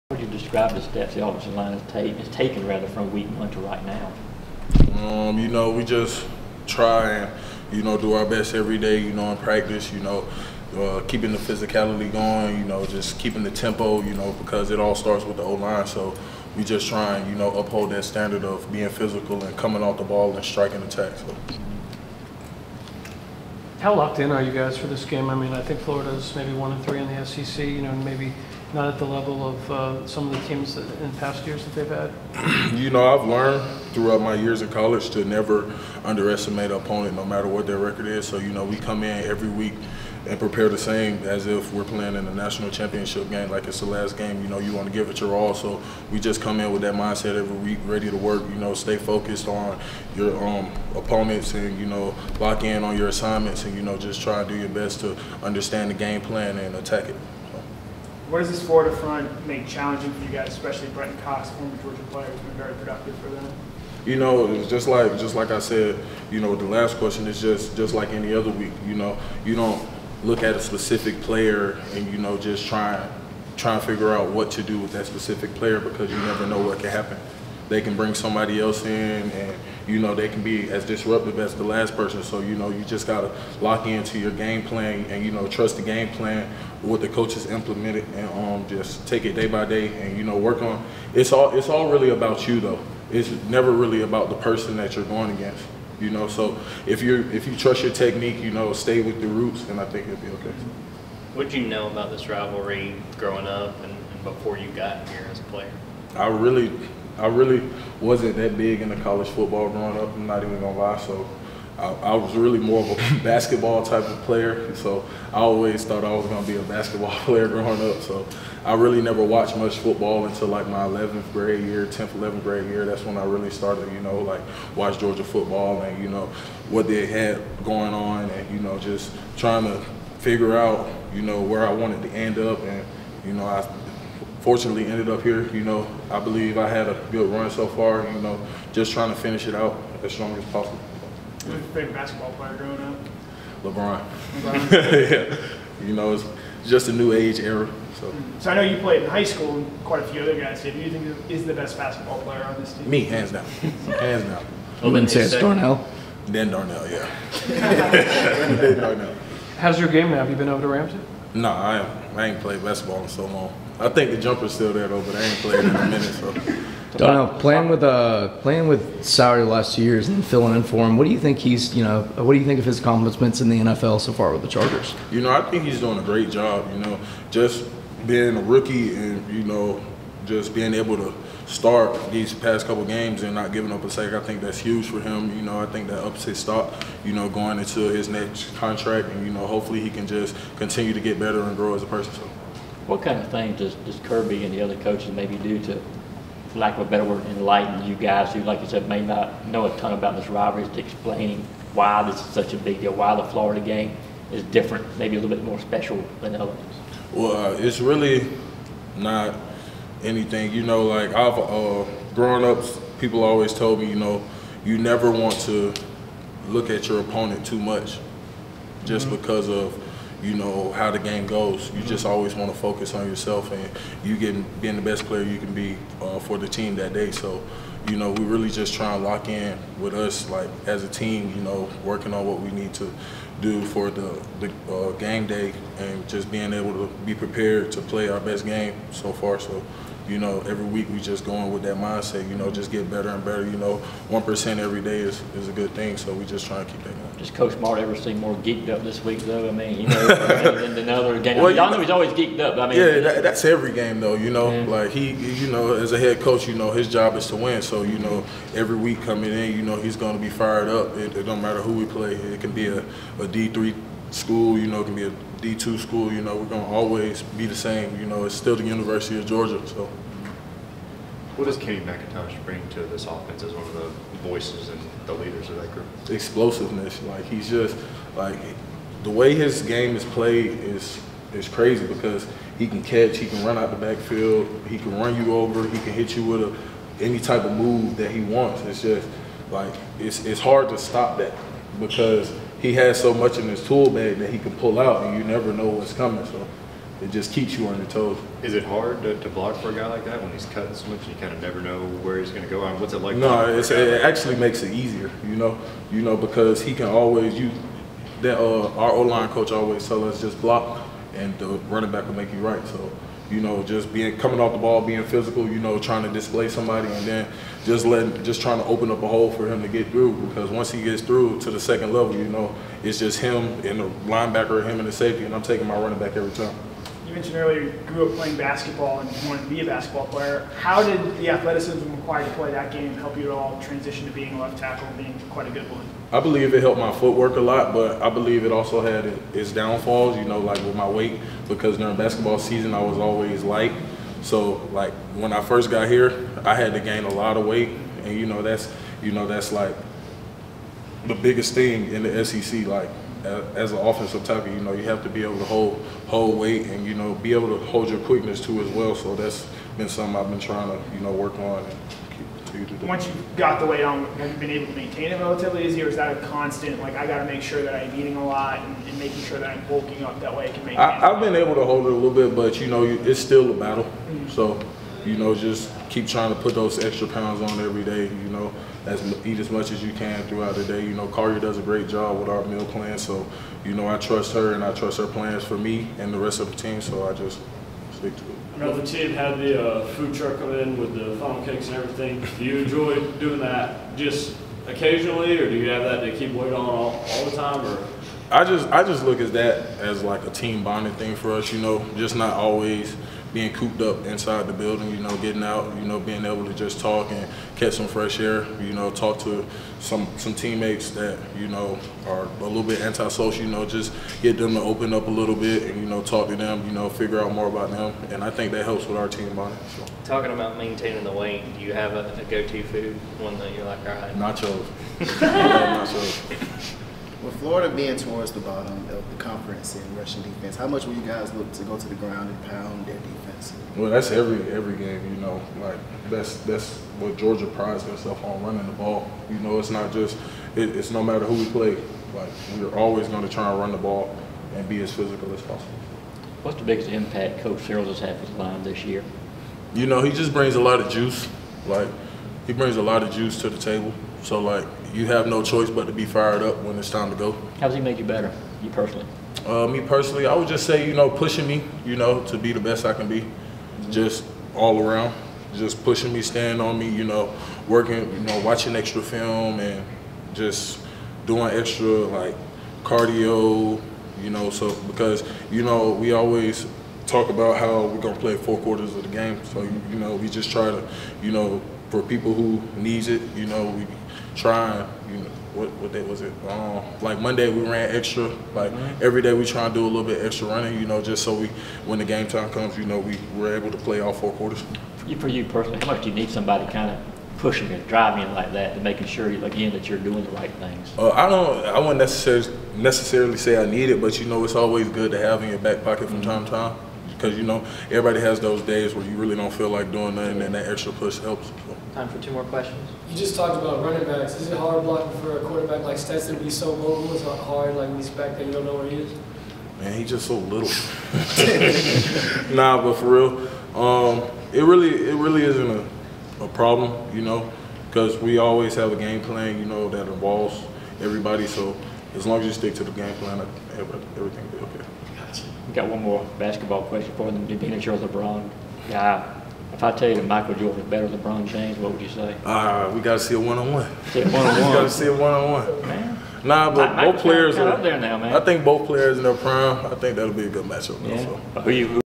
Broderick Jones on UGA OL coach Stacy Searels: "He brings the juice"
Follow along on Twitter @LanceMcCurley